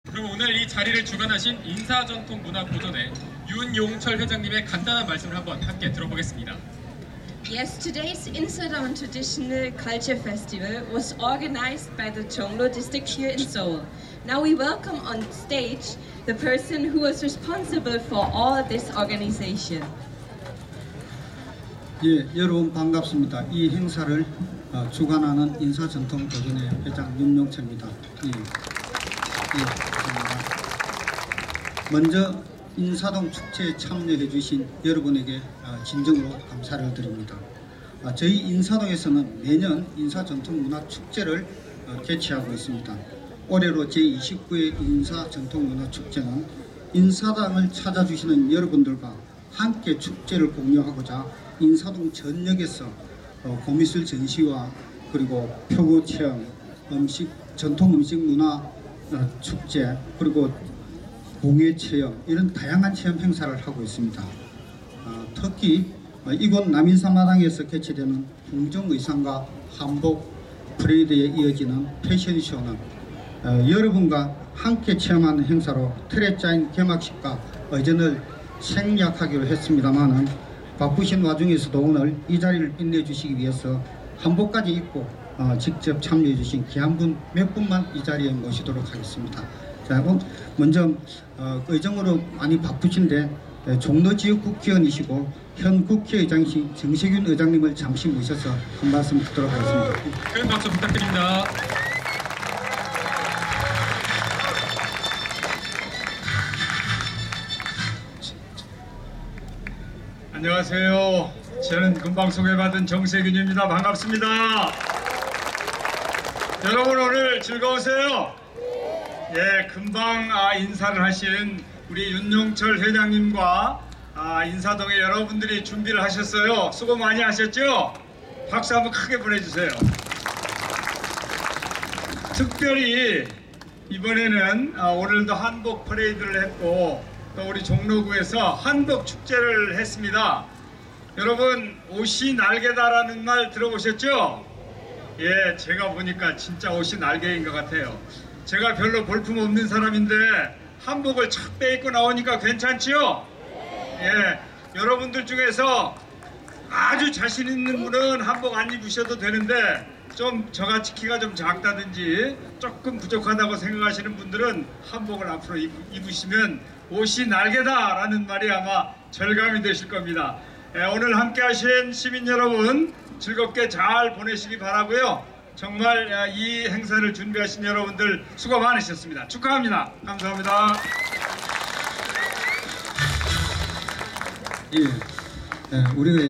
국회의장 정세균의 한복예찬, 제29회 인사전통문화축제 남인사마당 20160925
진행 연합뉴스 TV아나운서 이남규
이데일리TV 아나운서
인사동 외국인 홍보대사 윤안나